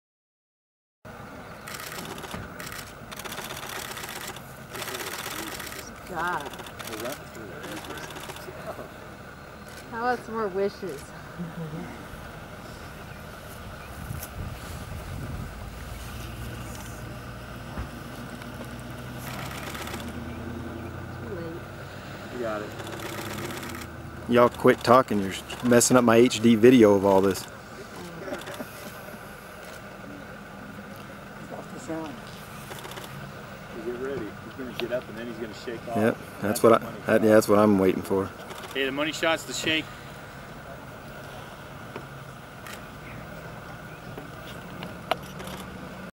Black Bear Swim
Black bear takes a swin in Floating Island Lake in Yellowstone National Park.  With the first warm temps of the year this seemed to be the right idea.